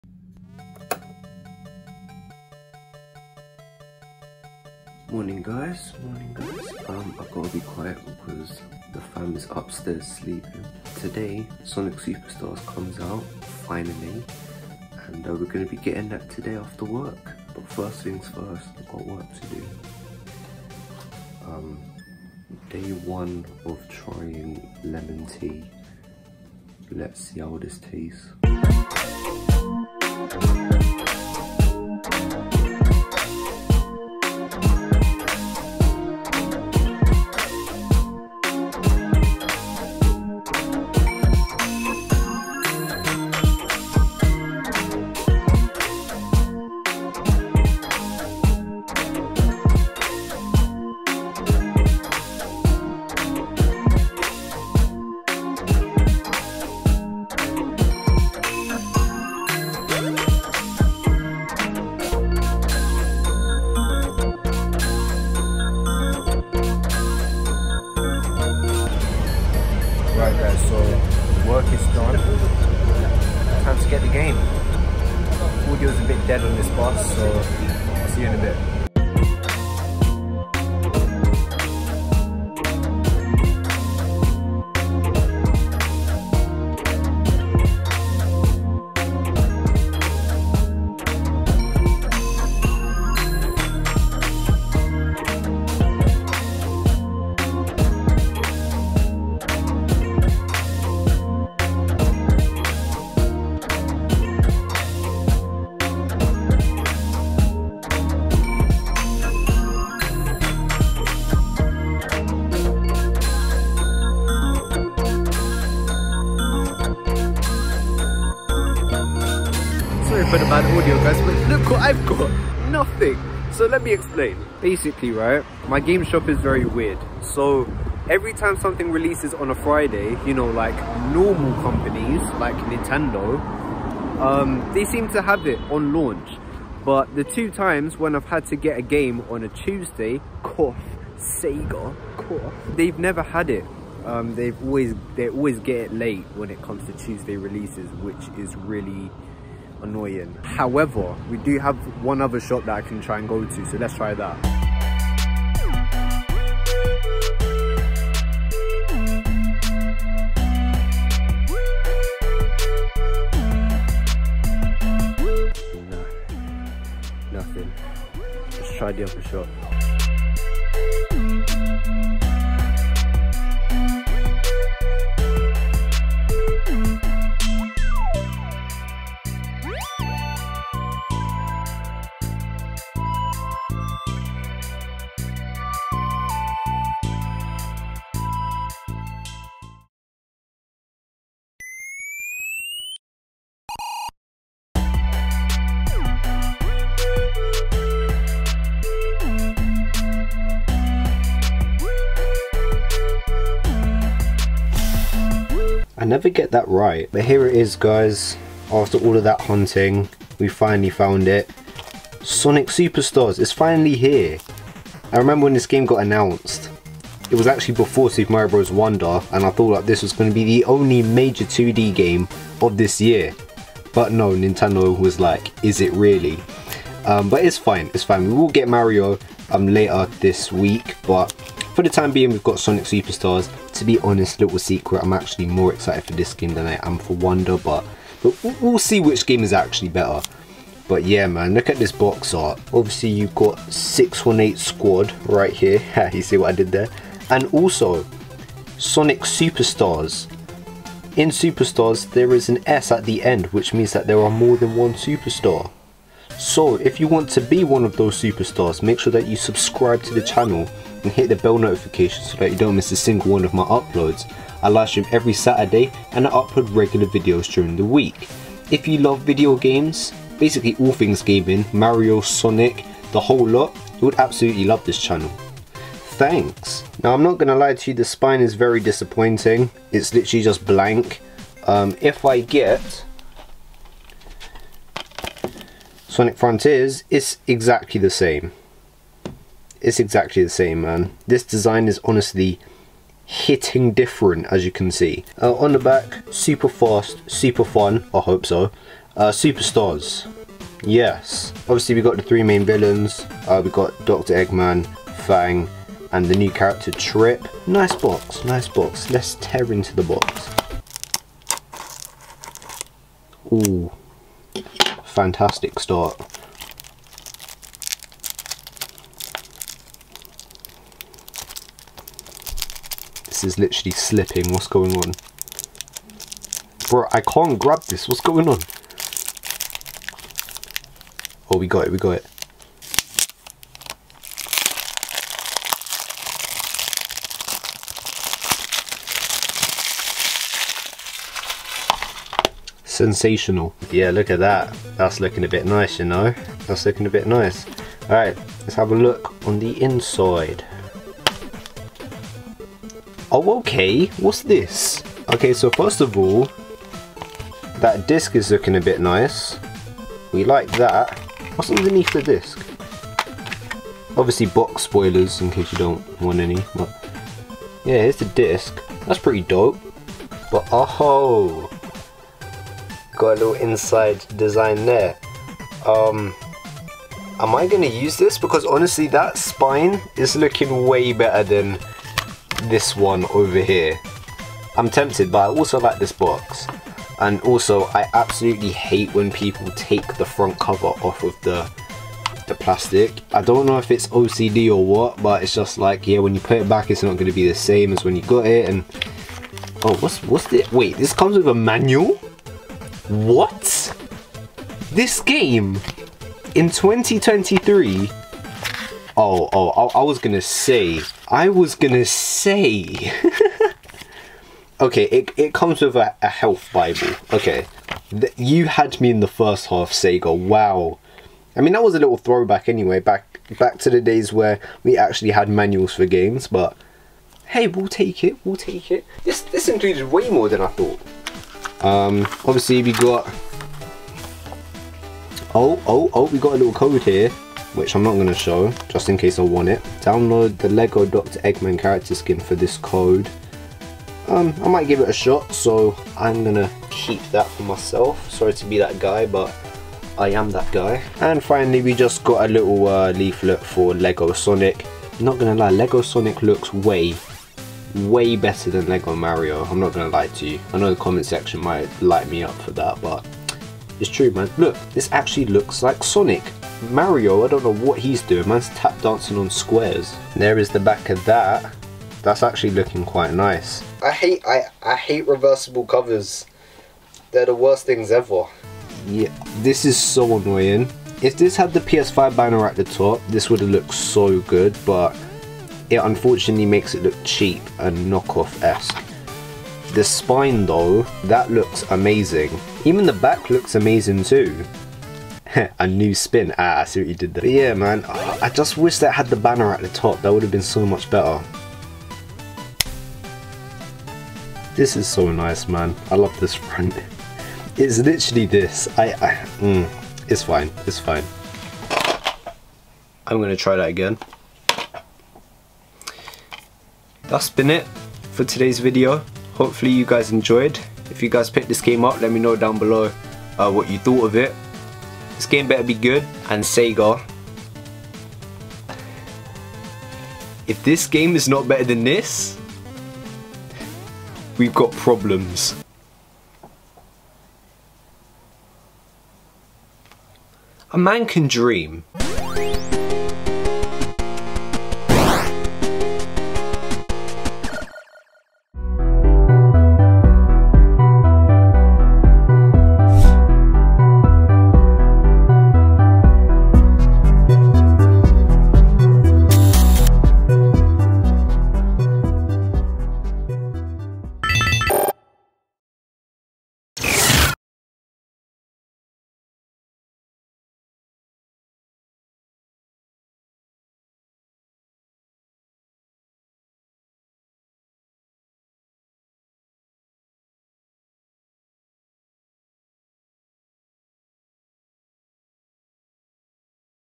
Unboxing & Vlogging Sonic Superstars on LAUNCH DAY
Hey everyone! Today I'll be Unboxing & Vlogging Sonic Superstars on LAUNCH DAY! Sonic Superstars is one of my most anticipated games for 2023 so its very exciting to have Sonic Superstars in my hands now! Sonic Superstars seems like a very fun game and a nice step forward to 2D Sonic games. I can't wait to see just how good Sonic Superstars is! But as for the video, WHAT A DAY. It was an adventure. All to obtain Sonic Superstars. Lets hope Sonic Superstars was worth the quest. Also, please do me a favour and comment down below if you did enjoy it, your honest feedback is very much appreciated and it helps me to learn and improve as a content creator. Hope you enjoy this short video!

Credits:

Thumbnail made by: The SGB
Edited by: The SGB

& SGBeatz: @sgbeatz618